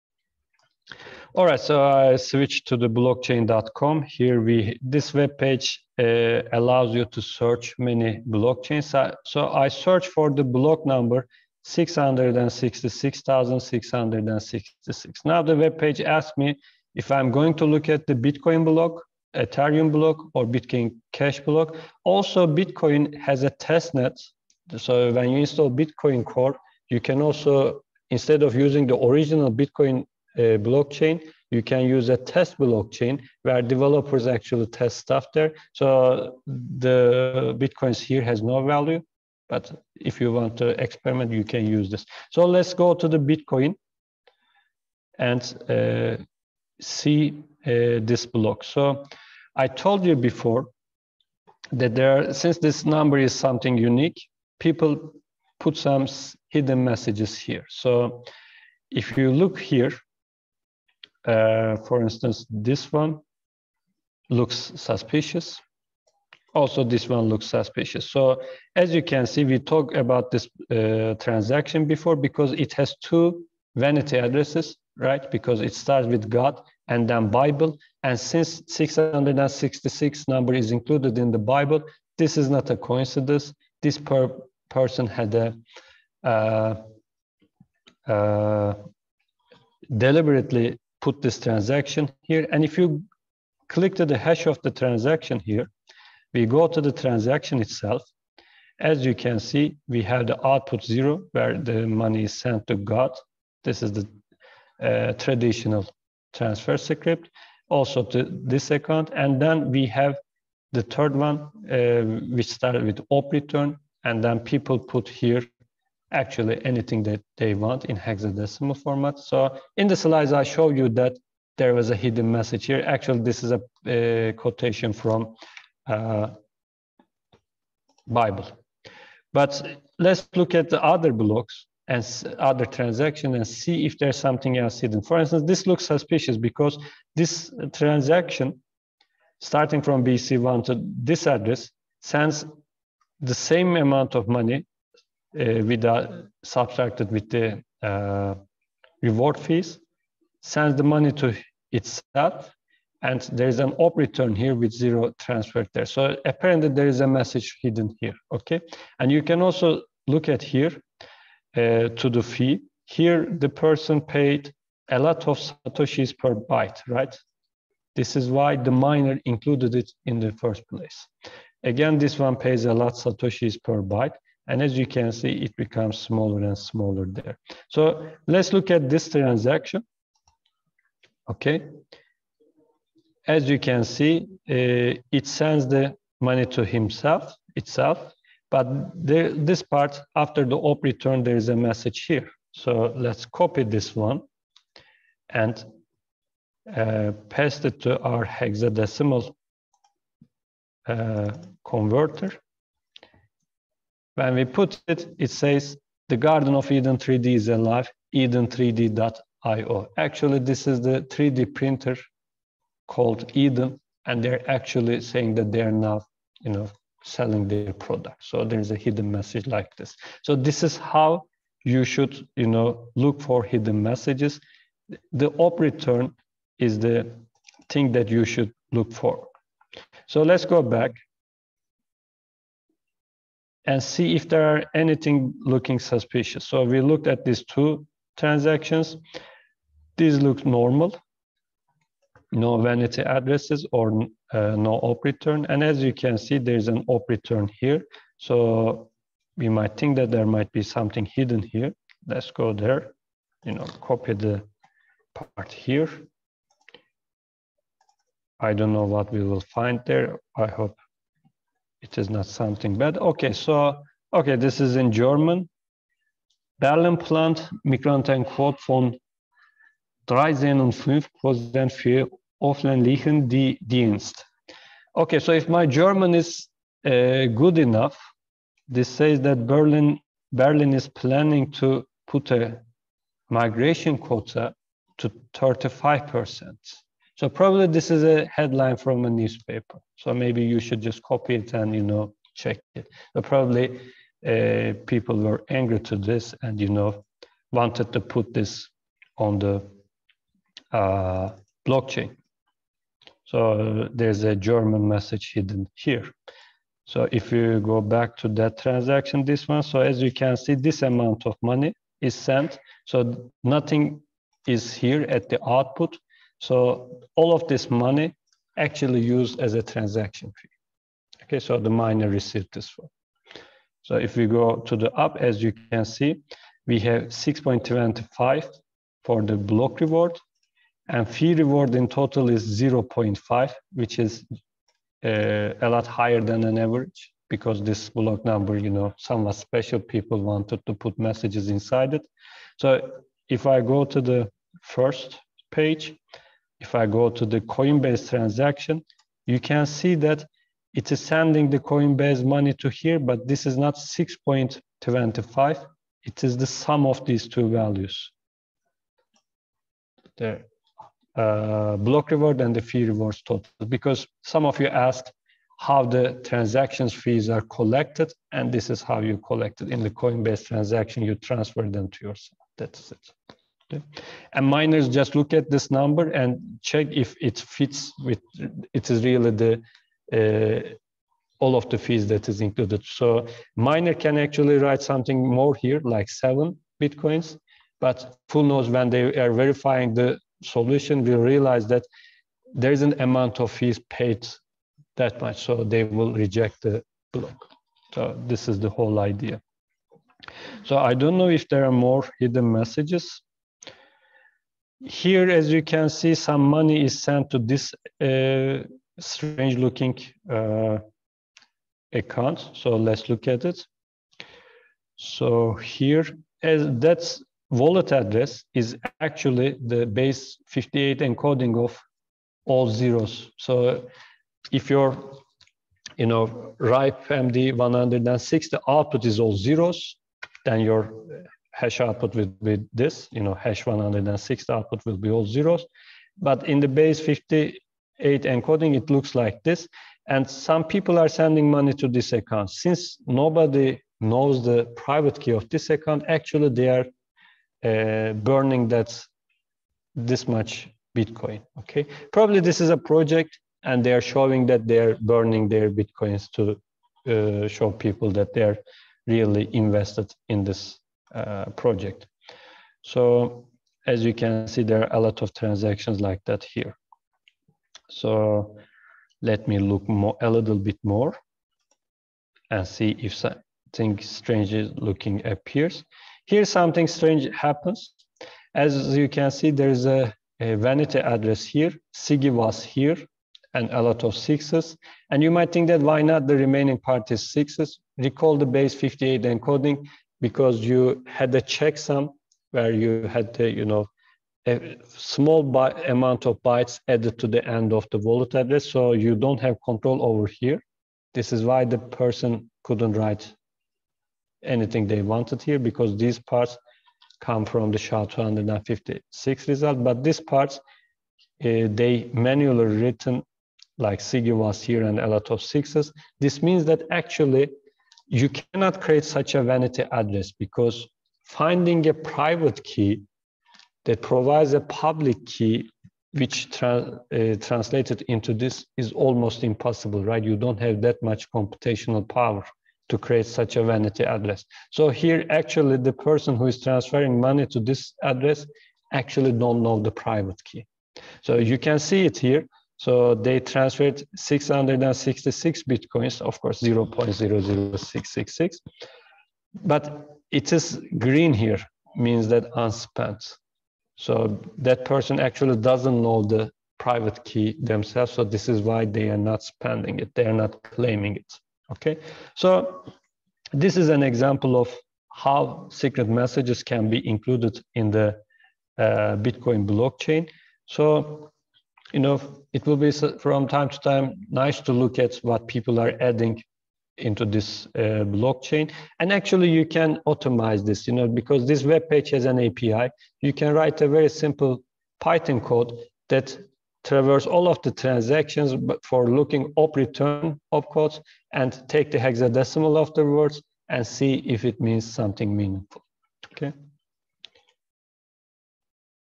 How to Find Hidden Messages in Bitcoin Blockchain
We investigate some transactions from the Bitcoin block 666,666 for hidden messages.